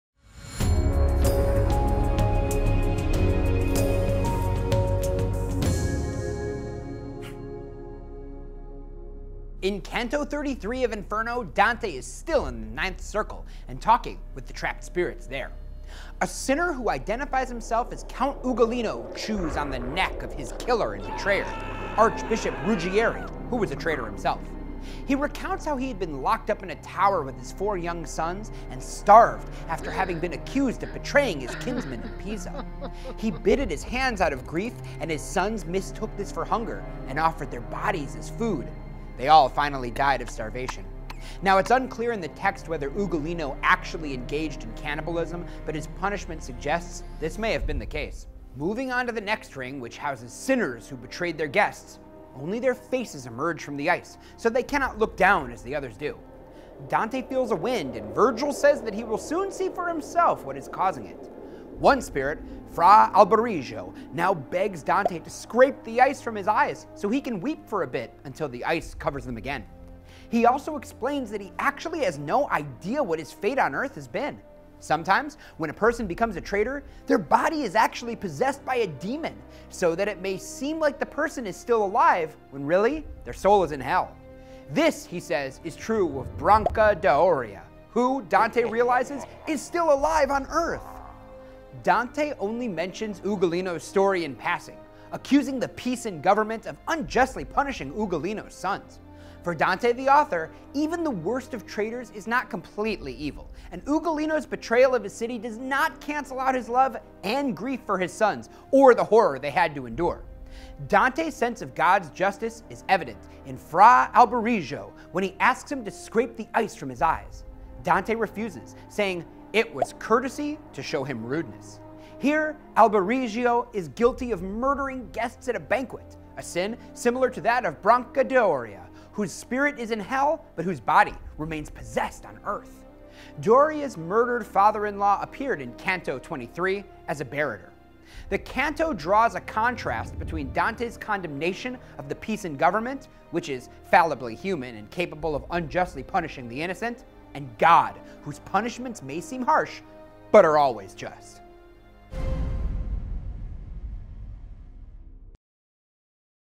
Dante's Inferno | Canto 33 Summary & Analysis
Summarize videos instantly with our Course Assistant plugin, and enjoy AI-generated quizzes Dante's Inferno summary in under five minutes!  Dante Alighieri's epic poem Inferno, the first part of Dante's Divine Comedy, is the classic Italian book about the nine circles of hell.  This video gives an in-depth summary and analysis of Canto 33 of Dante Alighieri's epic poem Inferno.

Download the free study guide and infographic for Dante Alighieri's epic poem Inferno